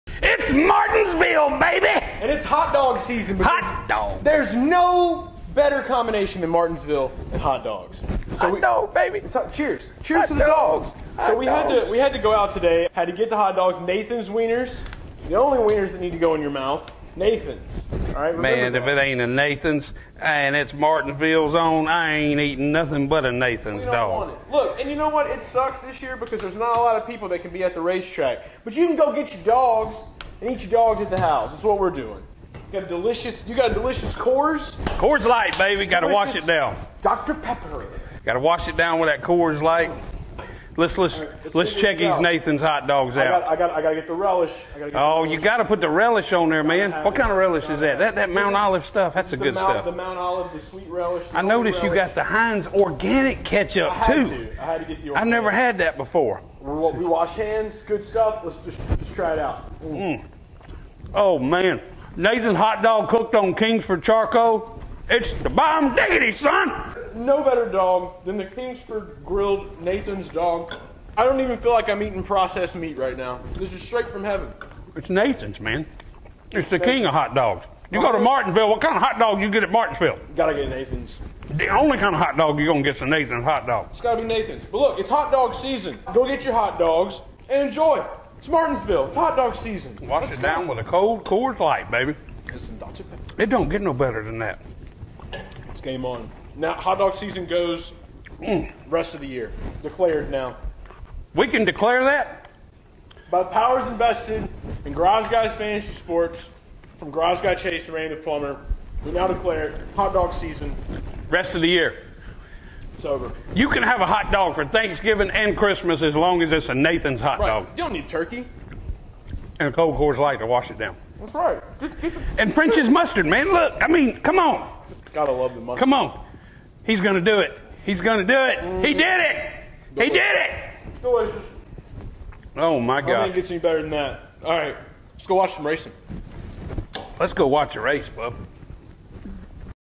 Garage Guy Chase and Randy The Plumber Announce Hot Dog Season
Garage Guy Chase and Randy The Plumber are watching the NASCAR race at Martinsville Speedway and officially announce the start of Hot Dog season in 2020. With Nathans Hot Dogs at hand, the make Joey Chestnut proud by showing you how to make the perfect Hot Dog by using Heinz Organic Ketchup, French's Mustard, and Mt. Olive Relish. Randy sips a cold Coors Light Beer and Chase sips on a crisp Dr. Pepper. The first NASCAR night race at Martinsville is a major success.